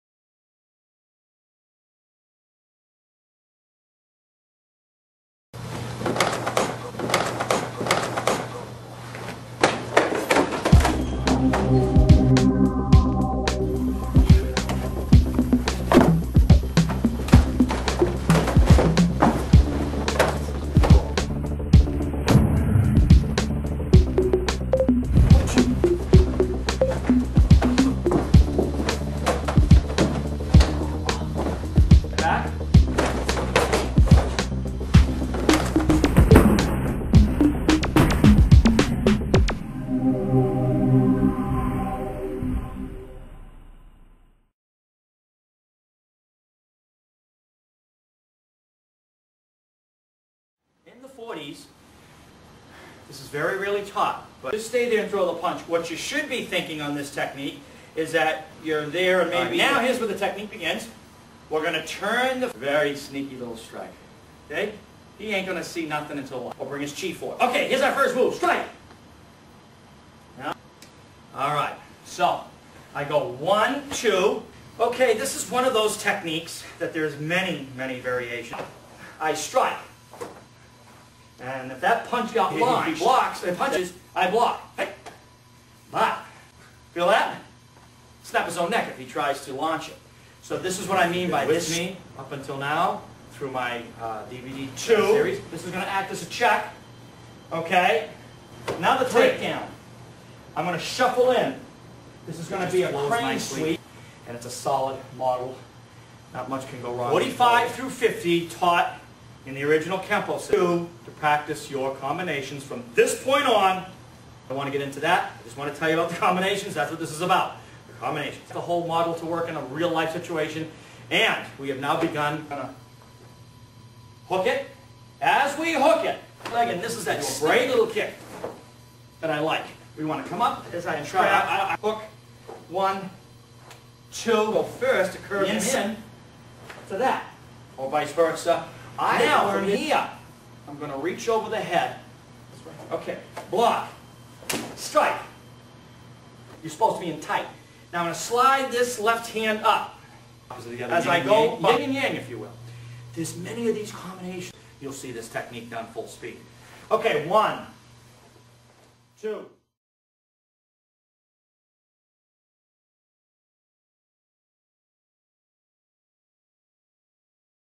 Shaolin Kempo Karate Kung-Fu 🐉🐉 Combination / DM 41-50 - GM Jim Brassard
Please give us your email for FREE tips and videos on the Martial Arts⬅️⬅️⬅️

Kempo Karate  The Original Mixed Martial Arts MMA.

Grandmaster James Brassard Karate kenpo techniques. Showing clips to the shaolin american kenpo karate combinations dvd/video.

The Kempo combinations have long been the bread and butter of the art. When I started in the mid 70s there was about 30 to 40, then over the years they progressed to 108. Each one contains Major and Minor principles that IF taught and extracted will give understanding to the Shaolin Kempo Arts.

With that being said, it could take a long time to understand just one of these techniques, which is not the purpose of these DVDs.

What is the purpose and benefit of these DVDs is to expose the many illogical movements that have mutated over the years.